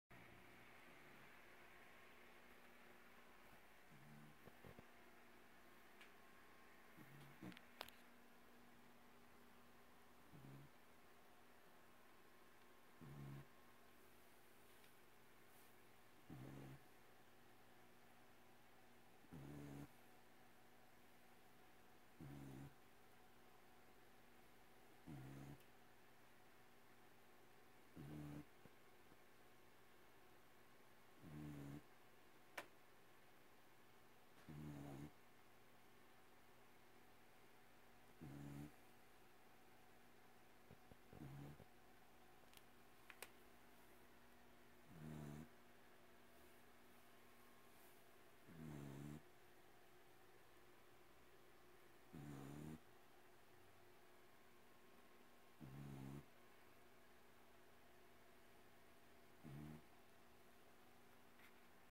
Do you think Abbie is in a good sleep?
She snores a lot, but what do you think she is dreaming about?